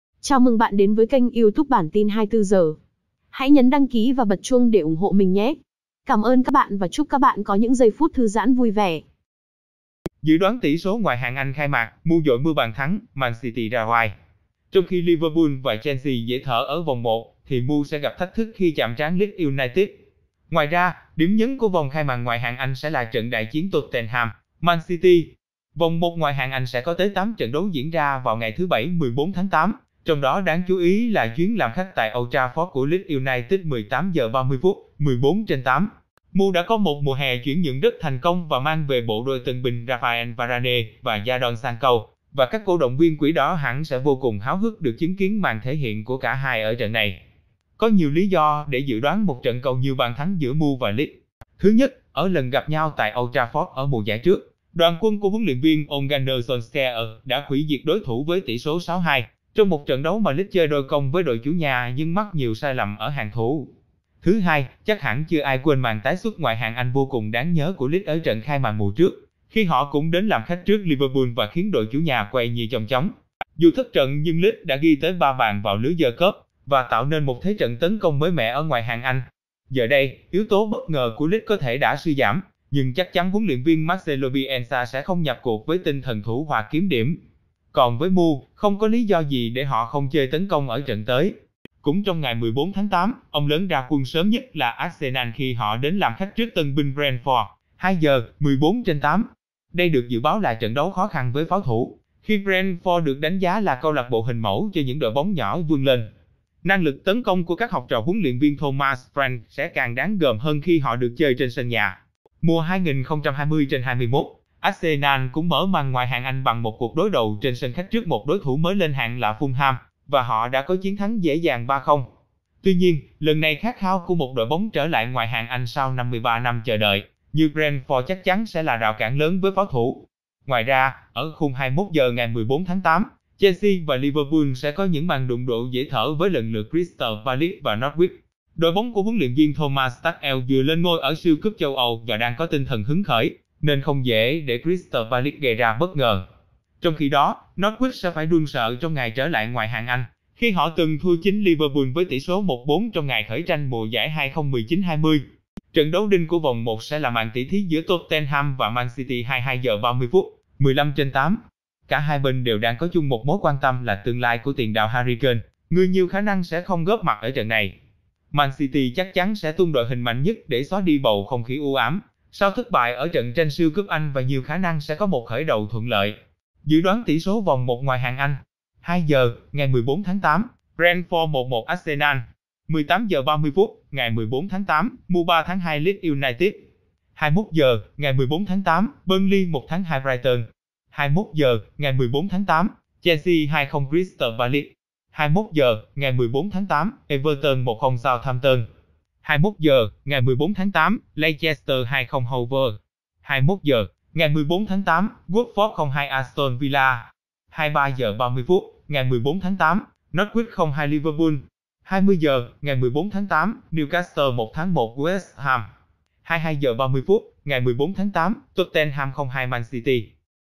Dự đoán tỉ số Ngoại hạng Anh khai mạc: MU dội "mưa bàn thắng", Man City ra oai
Trong khi Liverpool và Chelsea dễ thở ở vòng 1, thì MU sẽ gặp thách thức khi chạm trán Leeds United. Ngoài ra, điểm nhấn của vòng khai màn Ngoại hạng Anh sẽ là trận đại chiến Tottenham - Man City.
Vòng 1 Ngoại hạng Anh sẽ có tới 8 trận đấu diễn ra vào ngày thứ Bảy (14/8), trong đó đáng chú ý là chuyến làm khách tại Old Trafford của Leeds United (18h30, 14/8). MU đã có một mùa hè chuyển nhượng rất thành công và mang về bộ đôi tân binh Raphael Varane và Jadon Sancho, và các CĐV “Quỷ đỏ” hẳn sẽ vô cùng háo hức được chứng kiến màn thể hiện của cả hai ở trận này.
Có nhiều lý do để dự đoán một trận cầu nhiều bàn thắng giữa MU và Leeds. Thứ nhất, ở lần gặp nhau tại Old Trafford ở mùa giải trước, đoàn quân của HLV Ole Gunnar Solskjaer đã hủy diệt đối thủ với tỷ số 6-2, trong một trận đấu mà Leeds chơi đôi công với đội chủ nhà nhưng mắc nhiều sai lầm ở hàng thủ.
Thứ hai, chắc hẳn chưa ai quên màn tái xuất Ngoại hạng Anh vô cùng đáng nhớ của Leeds ở trận khai màn mùa trước, khi họ cũng đến làm khách trước Liverpool và khiến đội chủ nhà quay như chong chóng. Dù thất trận nhưng Leeds đã ghi tới 3 bàn vào lưới “The Kop” và tạo nên một thế trận tấn công mới mẻ ở Ngoại hạng Anh.
Giờ đây, yếu tố bất ngờ của Leeds có thể đã suy giảm, nhưng chắc chắn HLV Marcelo Bielsa sẽ không nhập cuộc với tinh thần thủ hòa kiếm điểm. Còn với MU, không có lý do gì để họ không chơi tấn công ở trận tới.
Cũng trong ngày 14/8, “ông lớn” ra quân sớm nhất là Arsenal khi họ đến làm khách trước tân binh Brentford (2h, 14/8). Đây được dự báo là trận đấu khó khăn với “Pháo thủ”, khi Brentford được đánh giá là CLB hình mẫu cho những đội bóng nhỏ vươn lên. Năng lực tấn công của các học trò HLV Thomas Frank sẽ càng đáng gờm hơn khi họ được chơi trên sân nhà.
Mùa 2020/21, Arsenal cũng mở màn Ngoại hạng Anh bằng một cuộc đối đầu trên sân khách trước một đối thủ mới lên hạng là Fulham, và họ đã có chiến thắng dễ dàng 3-0. Tuy nhiên, lần này khát khao của một đội bóng trở lại Ngoại hạng Anh sau 53 năm chờ đợi như Brentford chắc chắn sẽ là rào cản lớn với “Pháo thủ”.
Ngoài ra, ở khung 21h ngày 14/8, Chelsea và Liverpool sẽ có những màn đụng độ dễ thở với lần lượt Crystal Palace và Norwich.
Đội bóng của HLV Thomas Tuchel vừa lên ngôi ở Siêu cúp châu Âu và đang có tinh thần hứng khởi, nên không dễ để Crystal Palace gây ra bất ngờ. Trong khi đó, Norwich sẽ phải run sợ trong ngày trở lại Ngoại hạng Anh, khi họ từng thua chính Liverpool với tỉ số 1-4 trong ngày khởi tranh mùa giải 2019/20.
Trận đấu đinh của vòng 1 sẽ là màn tỉ thí giữa Tottenham và Man City (22h30, 15/8). Cả hai bên đều đang có chung một mối quan tâm là tương lai của tiền đạo Harry Kane, người nhiều khả năng sẽ không góp mặt ở trận này. Man City chắc chắn sẽ tung đội hình mạnh nhất để xóa đi bầu không khí u ám sau thất bại ở trận tranh Siêu cúp Anh và nhiều khả năng sẽ có một khởi đầu thuận lợi.
Dự đoán tỉ số vòng 1 Ngoại hạng Anh
2h, 14/8: Brentford 1-1 Arsenal
18h30, 14/8: MU 3-2 Leeds United
21h, 14/8: Burnley 1-2 Brighton
21h, 14/8: Chelsea 2-0 Crystal Palace
21h, 14/8: Everton 1-0 Southampton
21h, 14/8: Leicester 2-0 Wolves
21h, 14/8: Watford 0-2 Aston Villa
23h30, 14/8: Norwich 0-2 Liverpool
20h, 15/8: Newcastle 1-1 West Ham
22h30, 15/8: Tottenham 0-2 Man City